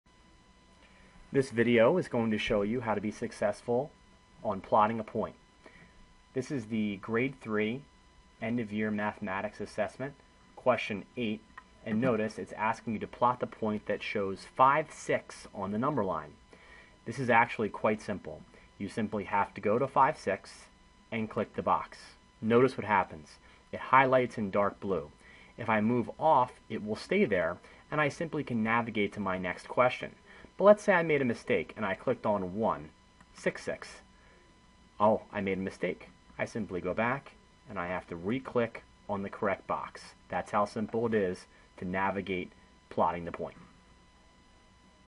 Plot a Point
This is going to be a video explaining how to be successful on different components of the PARCC Mathematics Sample End of Year Assessments.  This specific video will discuss how to properly plot a point.